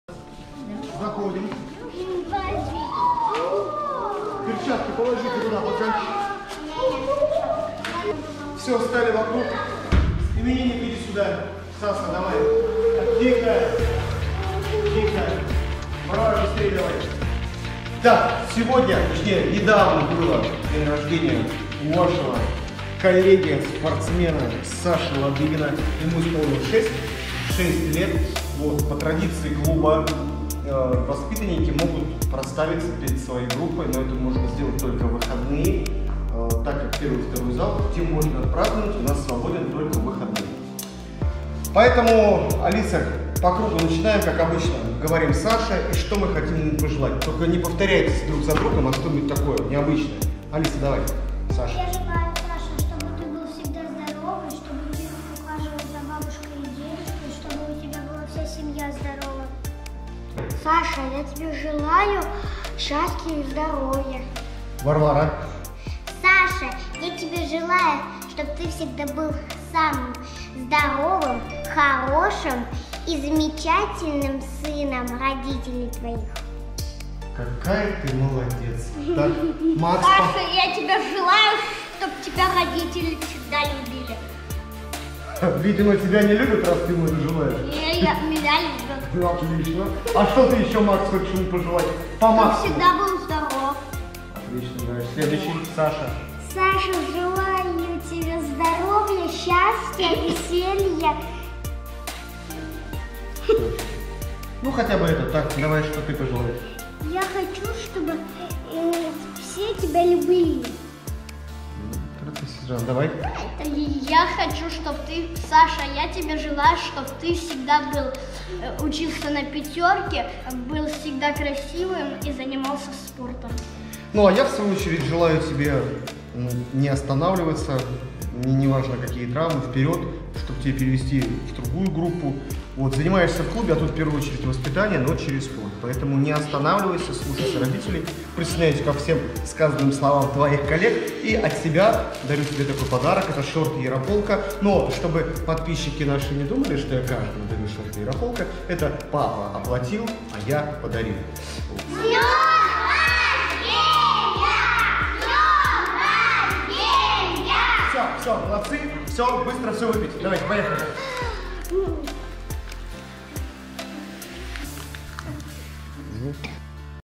Праздник "День рождения" в СПК "ЯРОПОЛК", город Красногорск, Ладыгин Александр, апрель 2021 г.
По традиции СПК "ЯРОПОЛК" любой желающий спортсмен клуба может накрыть праздничный стол для своих сверстников в честь "Дня рождения".
В апреле 2021 г.  его отметил Ладыгин Александр, воспитанник группы  5-6 лет, ему исполнилось 6 лет.